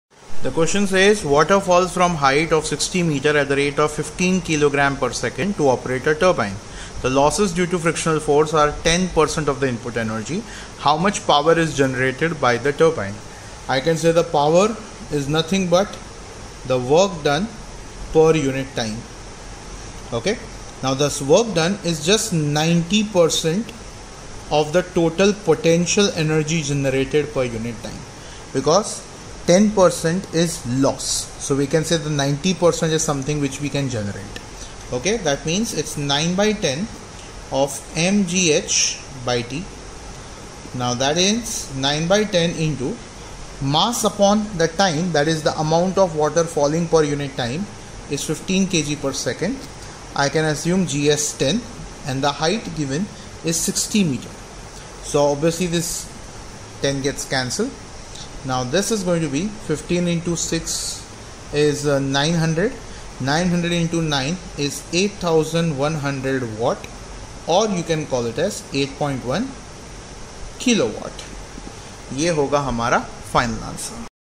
Water falls from height of 60 m at the rate of 15 kg/s to operate a turbine | NEET 2021 Solutions
Water falls from height of 60 m at the rate of
15 kg/s to operate a turbine. The losses due to
frictional force are 10% of the input energy.
How much power is generated by the
turbine? (g = 10m/s2)
a) 10.2 kW b) 8.1 kW
c) 12.3 kW d) 7.0 kW